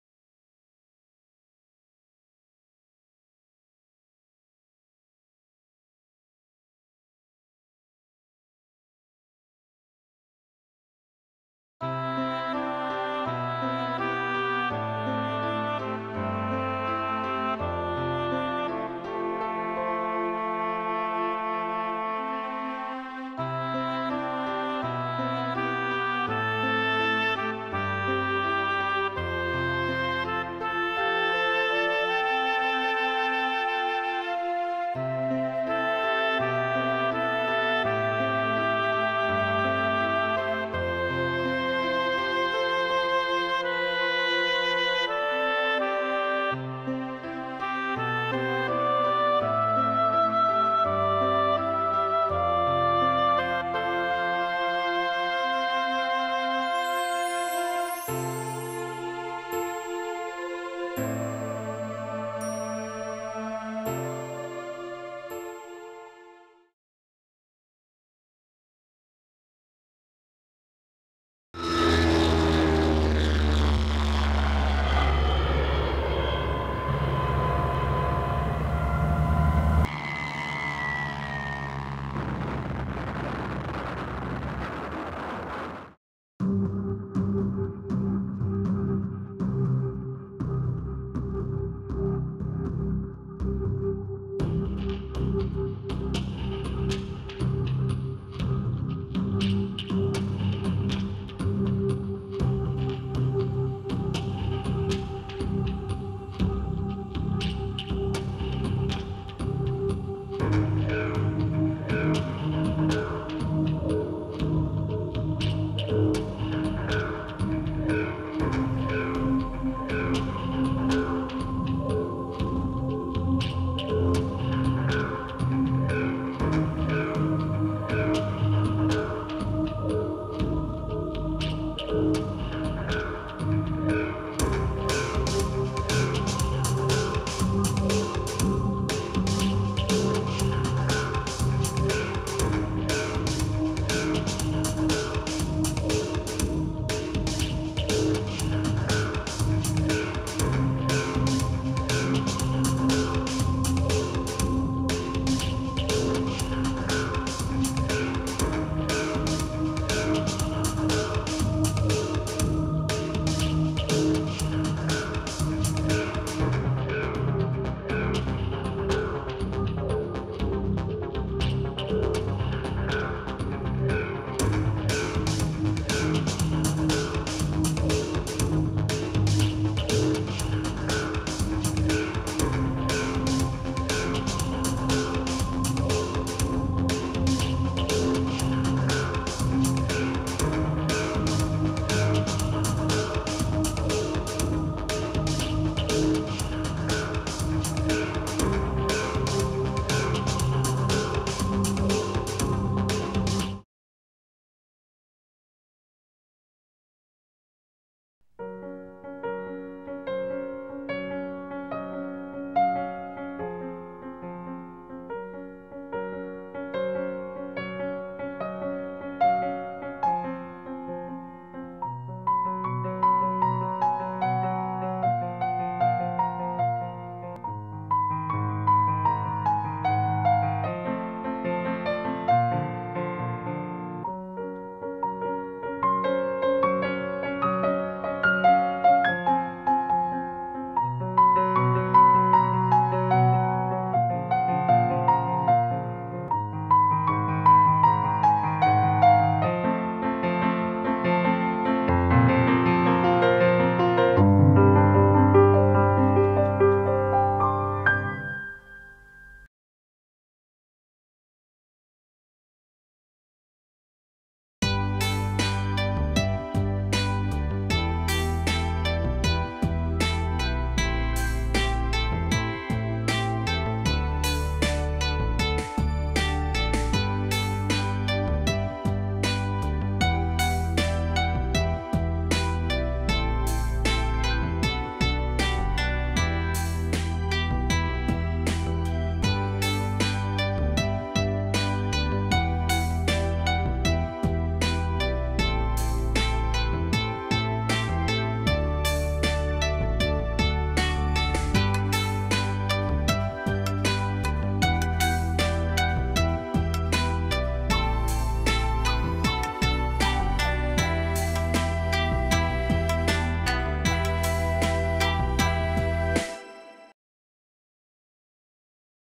岡山城今昔｜岡山映像ライブラリーセンター
2022年11月にリニューアルした金烏城…岡山城
RSKに保存されている映像の中から
戦災で焼失する前と再建された直後の岡山城の姿をまとめました。